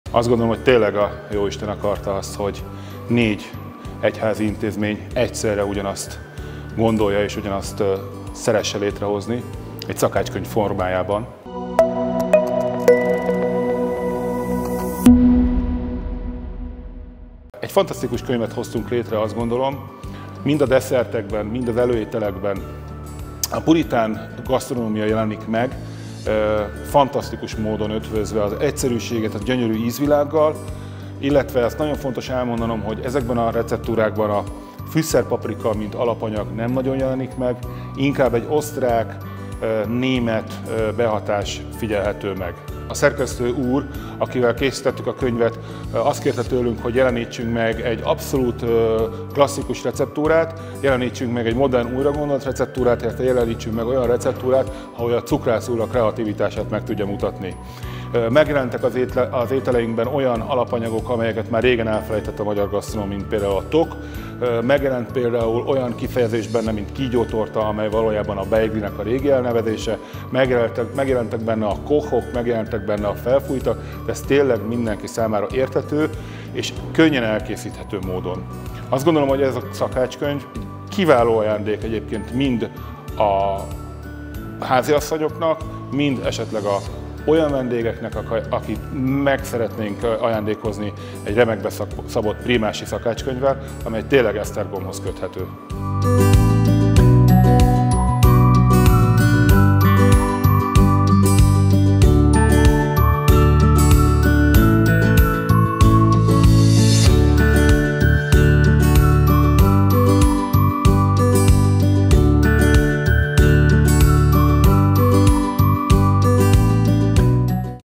Jászai Tibor - Ételek a prímási asztalról
Ételek a prímási asztalról címmel kötet jelent meg, melyről Jászai Tibort a Prímás Pince Étterem - konyhafőnökét kérdeztük.

A múlt ízeit kóstolhattak meg a vendégek az esztergomi Prímás Pincében. Még pedig azokat az ételeket, amelyek egykor a 19. századi prímások asztalára kerültek.